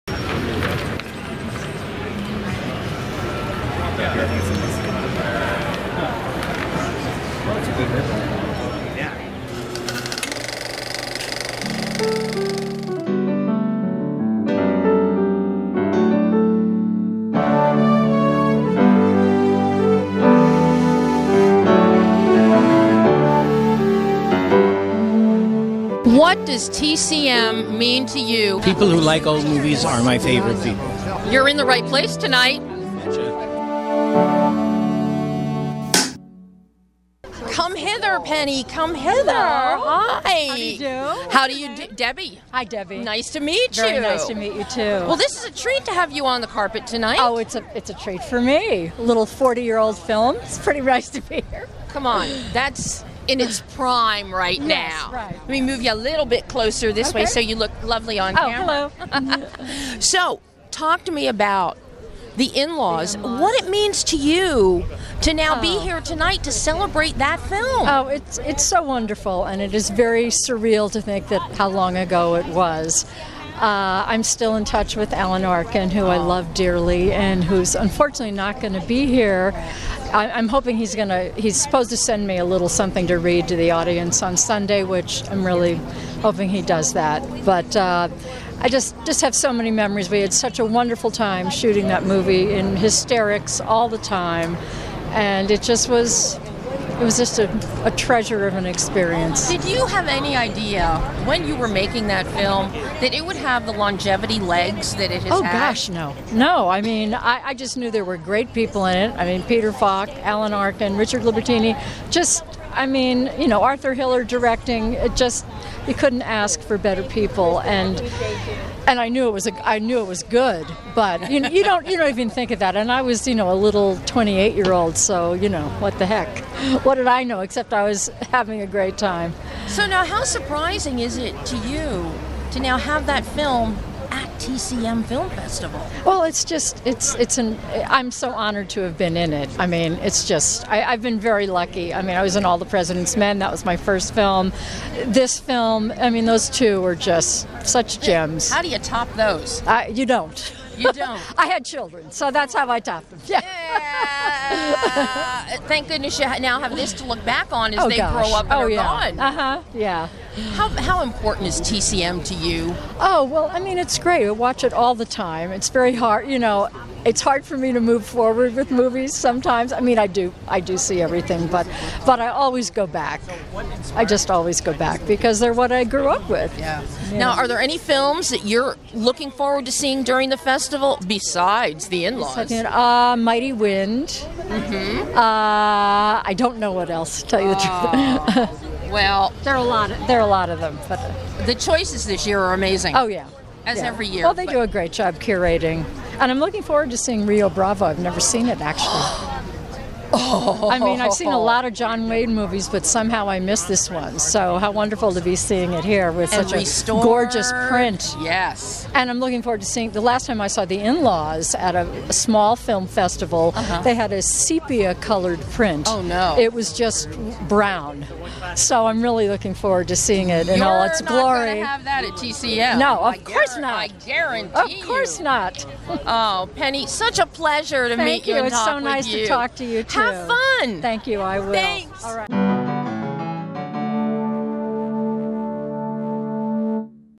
2023 TCM Classic Film Festival Carpet Chat with PENNY PEYSER
BEHIND THE LENS hit the red carpet for the 2023 TCM Classic Film Festival and a Carpet Chat with actor PENNY PEYSER.

Raindrops may have been falling on our heads during the TCM Classic Film Festival red carpet this April 13th, but the stars were shining brightly as they made their way down the carpet to the TCL Chinese Theatre for the Opening Night celebration and a screening of a true classic film - RIO BRAVO - complete with one the film's stars, ANGIE DICKINSON, in attendance.

And we're not just celebrating another TCM Classic Film Festival.  With a theme of "You Ain't Seen Nothing Yet", along with countless anniversary screenings and special guests, TCMFF is also celebrating the 100th anniversary of Warner Bros. Studios!

The TCM Classic Film Festival runs April 13-16, 2023 in Hollywood.